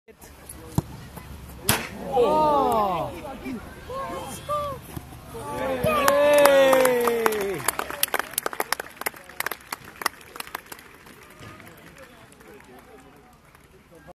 Pavel Srnicek Charity Match Cup - NUFC
Keith Gillespie grabbing a goal at today's Charity match at Dunston FC.